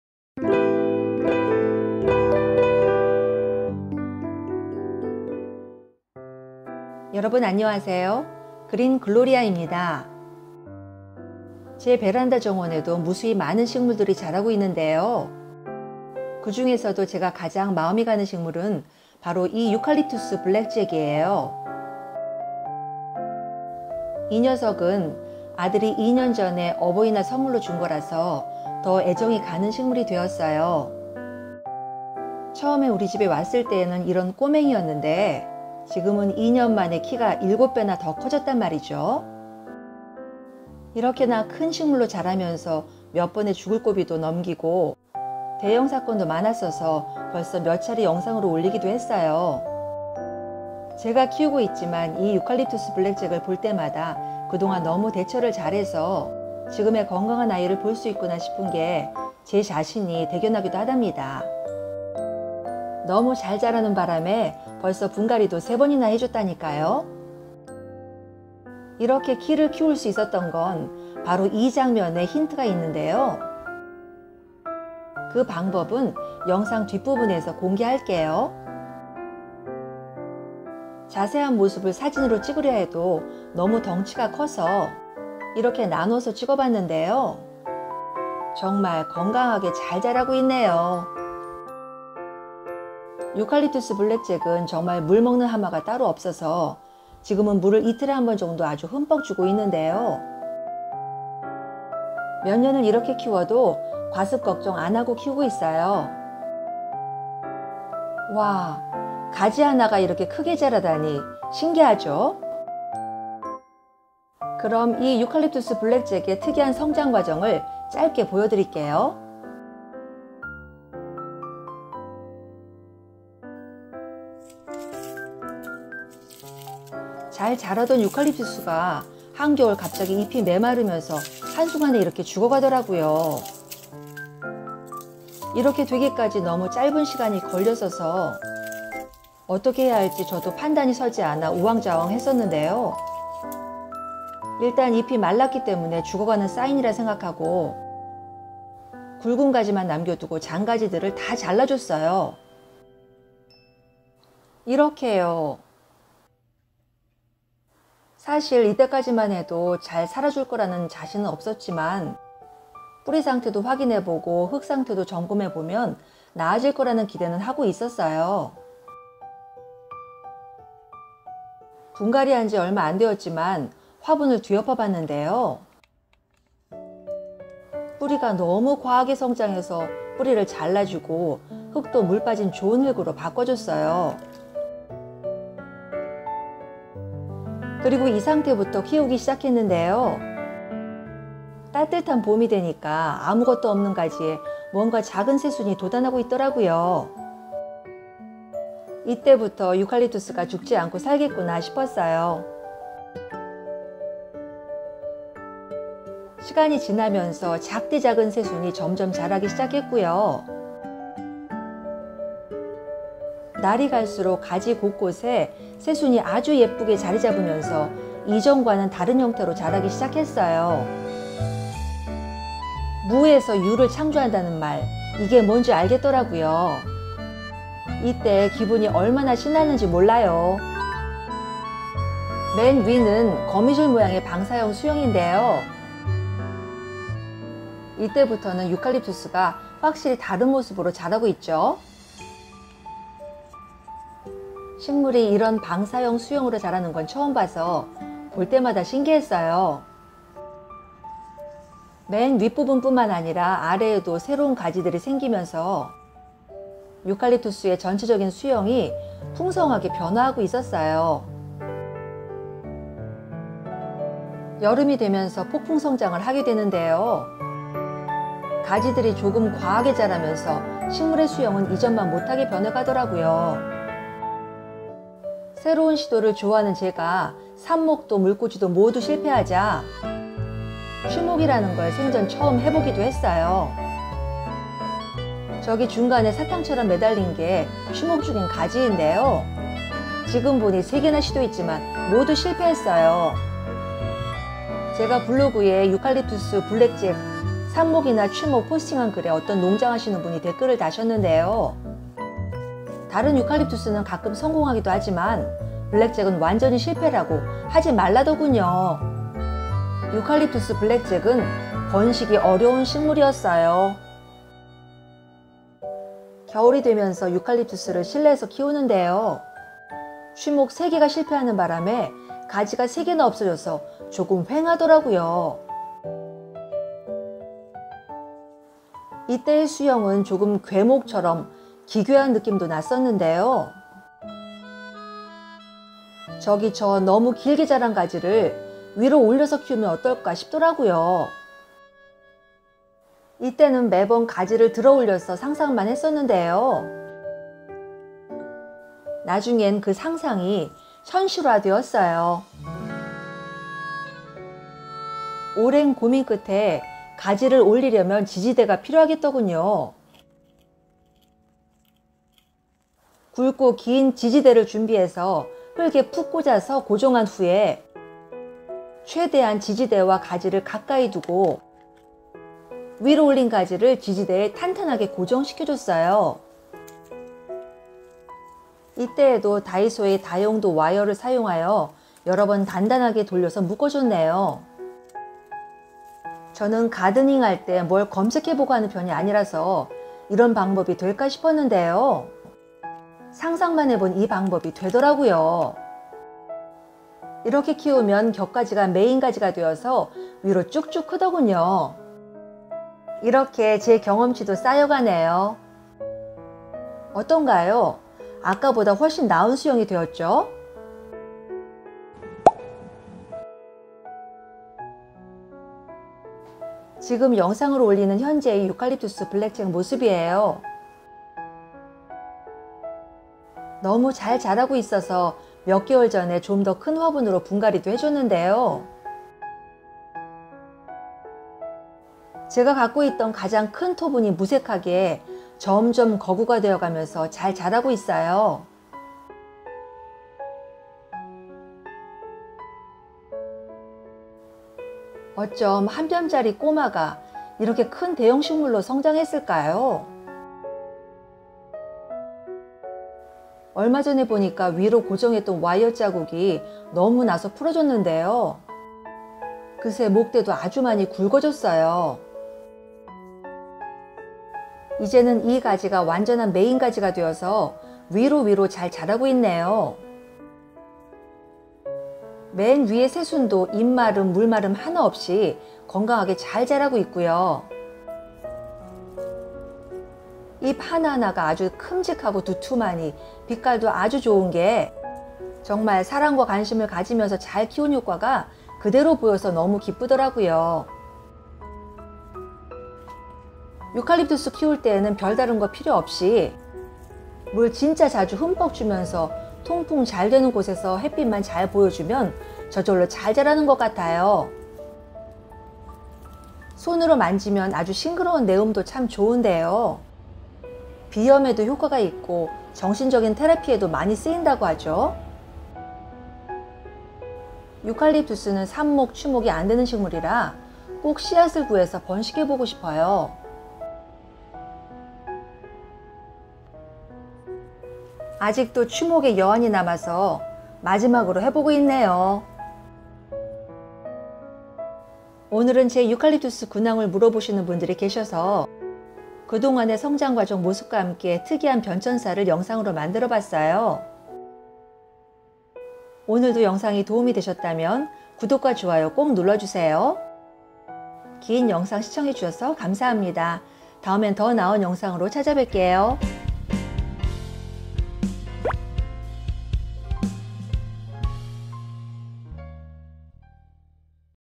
죽을 고비 넘긴 유칼립투스 현황 & 살려낸 비법 공개
유칼립투스는 실제로 키우기 만만치 않은 식물인데요, 또 오래 키우다 보면 나름의 노하우가 생겨서 그렇게 키우기 어렵지만은 않은 식물이기도 해요!
물주기가 꽤나 잦은 식물인데요, 물주기 텀을 잠깐 놓치면 바로 회생불가가 되기도 하죠!
그 어려운 유칼립투스도 골든 타임을 잘 지키면 살려낼 방법이 있었어요!
또한 수형 역시 내 마음대로 바꿀 수도 있는데요, 영상을 통해서 확인해주세요!

블로그에서도 자세한 내용 확인하세요~